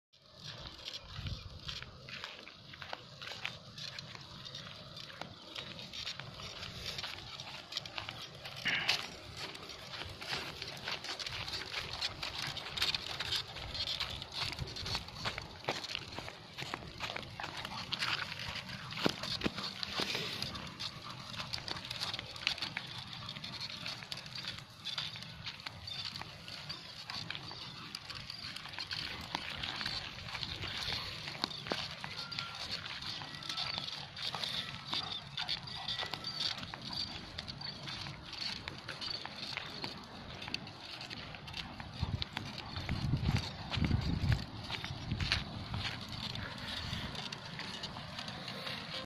Barney's daily morning walk on his wheel chair
The second week in his wheel chair